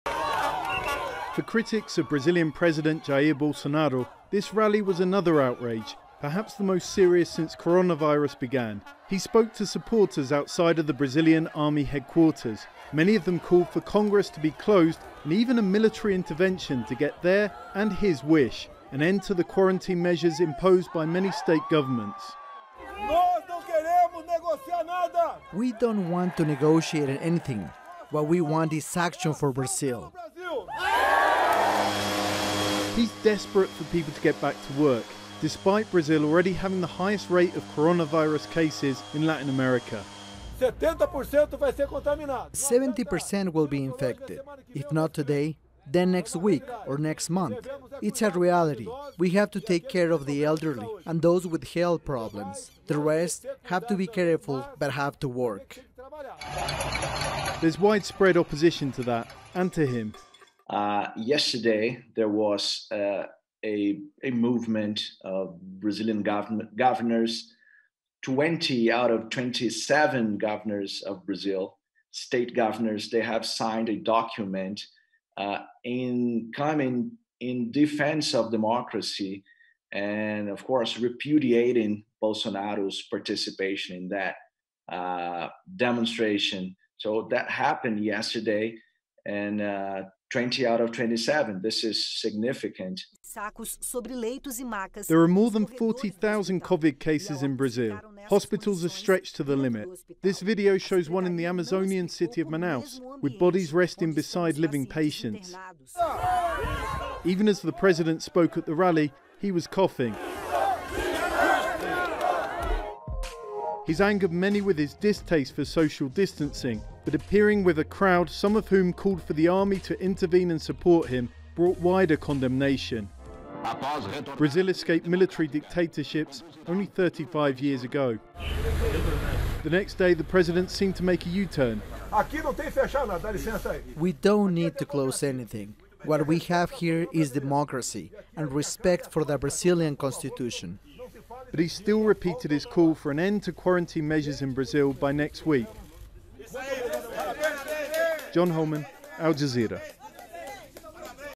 Brazil's Bolsonaro joins protest against coronavirus restrictions
Brazil has more than 40,000 infections and 2,500 deaths from the coronavirus pandemic, making it the worst-affected country in Latin America.
But President Jair Bolsonaro has been eager to get the country back on its feet economically.
On Sunday he joined protesters calling for the virus restrictions to be lifted and said the military should intervene.
But the governors who imposed these curbs have denounced Bolsonaro's actions.

Al Jazeera’s John Holman reports.